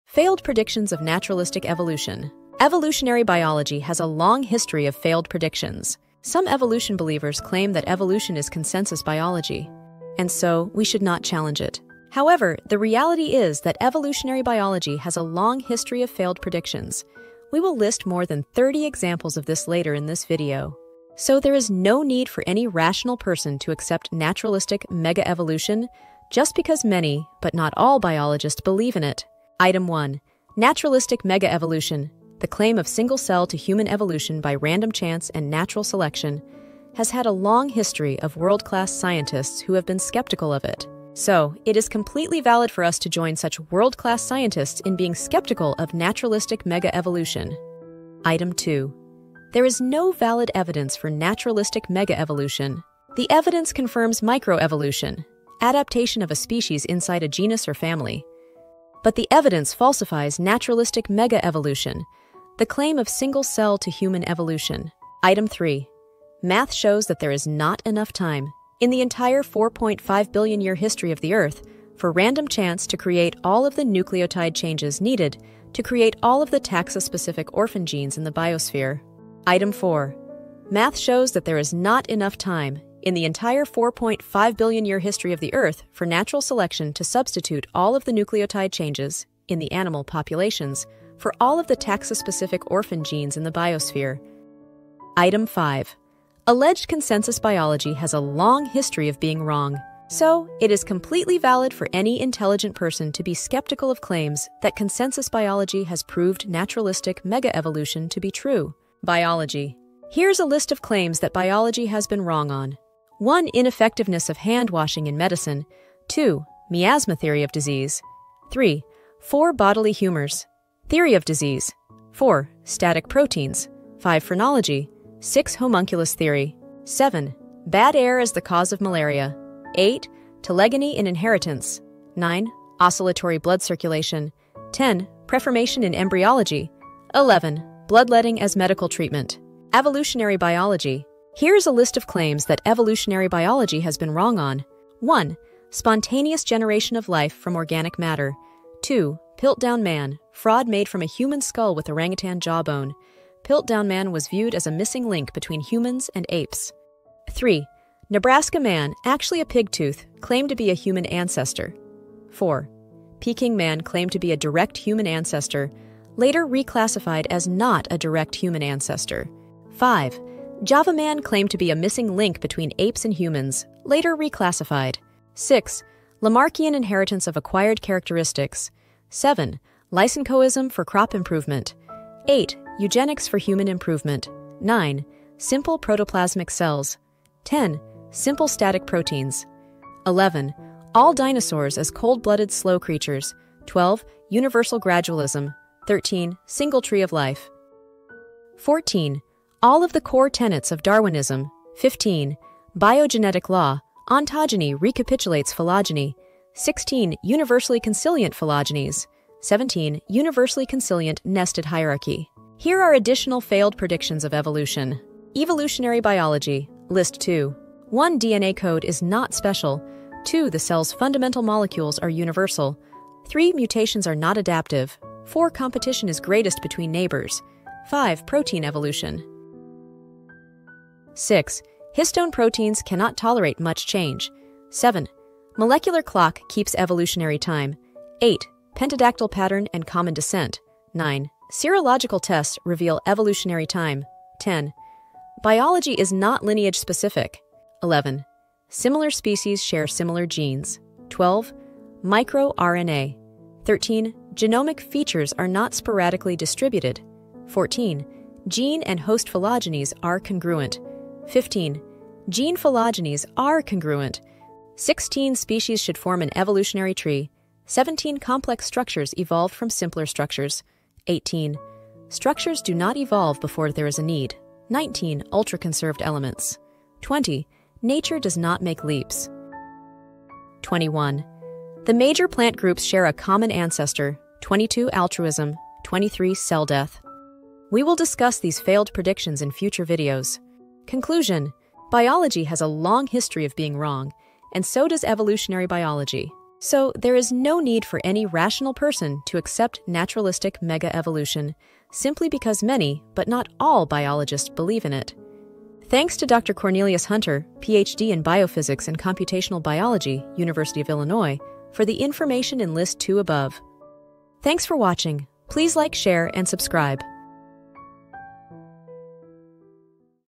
Failed Predictions of Naturalistic Evolution
In this thought-provoking video, we delve into the realm of evolutionary biology and examine its long history of failed predictions. Contrary to the notion of Evolution being consensus biology, this video highlights more than 30 examples of predictions that didn't stand the test of time.

We explore key items such as the skepticism surrounding Naturalistic Mega-Evolution, the lack of valid evidence supporting it, and the mathematical challenges it faces in explaining the complexity of life on Earth. The video also draws parallels with past instances where biology, considered factual at the time, eventually proved to be incorrect.

From the ineffective hand washing in medicine to the infamous Piltdown Man hoax, we draw a parallel between historical biology misconceptions and the uncertainties in evolutionary biology. We also discuss additional failed predictions within Evolutionary Biology, shedding light on the challenges it faces in explaining the diversity of life.

Join us as we question widely held beliefs and engage in a critical examination of Evolutionary Biology. Whether you're a skeptic or a believer, this video provides valuable insights into the ongoing debate. Special thanks to Dr. Cornelius Hunter for his valuable contributions.

EVOLUTIONARY BIOLOGY: Here is a list of claims that Evolutionary Biology has been wrong on. (1) Spontaneous generation of life from organic matter; (2) Piltdown Man, fraud made from a human skull with orangutan jawbone; Piltdown man was viewed as a missing link between humans and apes. (3) Nebraska Man, actually a pig tooth, claimed to be a human ancestor; (4) Peking Man claimed to be a direct human ancestor, later reclassified as NOT a direct human ancestor. (5) Java man claimed to be a missing link between apes and humans, later reclassified. (6) Lamarckian inheritance of acquired characteristics; (7) Lysenkoism for crop improvement; (8) Eugenics for human improvement, (9) Simple protoplasmic cells; (10) simple static proteins. (11) All Dinosaurs as cold-blooded slow creatures; (12) Universal Gradualism; (13) Single Tree of Life; (14) ALL of the core-tenets of Darwinism; (15) Biogenetic law, ontogeny recapitulates phylogeny; (16) Universally consilient phylogenies; (17) universally consilient nested hierarchy. Here are additional FAILED predictions of Evolution. EVOLUTIONARY BIOLOGY (list 2). (1) DNA code is not special; (2) The Cell’s fundamental molecules are universal; (3) Mutations are not adaptive; (4) Competition is greatest between neighbors; (5) Protein Evolution. (6) Histone proteins cannot tolerate much change; (7) Molecular clock keeps evolutionary time; (8) Pentadactyl pattern and common descent; (9) Serological tests reveal evolutionary time; (10) Biology is not lineage specific. (11) Similar species share similar genes; (12) Micro RNA; (13) Genomic features are not sporadically distributed; (14) gene and host phylogenies are congruent; (15) Gene phylogenies are congruent. (16) Species should form an evolutionary tree; (17) complex structures evolved from simpler structures; (18) Structures do not evolve before there is a need; (19) ultra-conserved elements; (20) nature does not make leaps. (21) The major plant groups share a common ancestor; (22) altruism; (23) cell death. We will discuss these failed predictions in future videos. CONCLUSION: Biology has a LONG history of being wrong; and so does Evolutionary Biology. So, there is NO need for any rational person to accept Naturalistic Mega-Evolution simply because many (but not all) biologists believe in it.

Thanks to Dr Cornelius Hunter (PhD in Biophysics and Computational Biology, University of Illinois) for the information in list 2 above.